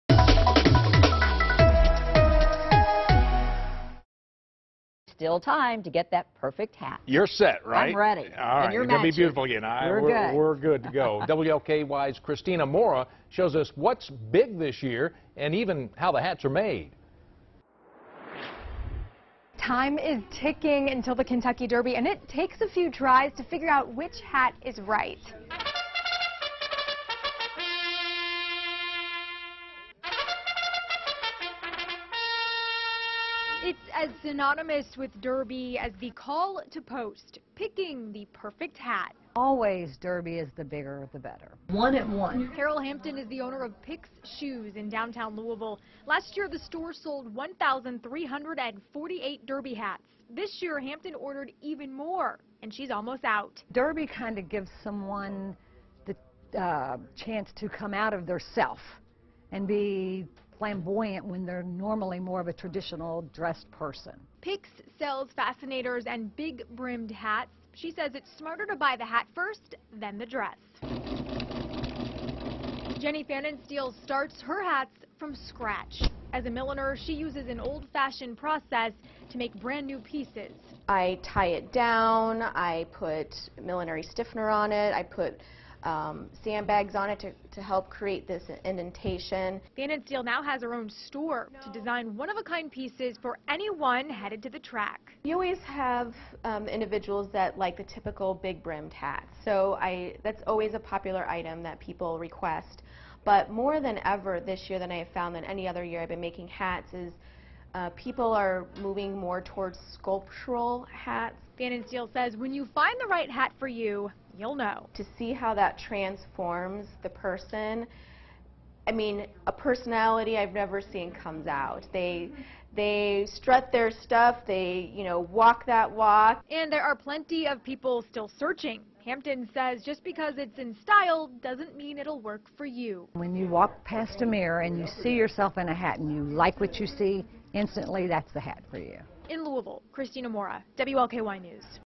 Experts offer last-minute Oaks and Derby hat ideas
Still looking for that perfect hat? Time's running out, but it can be found. WLKY's Christina Mora has the story.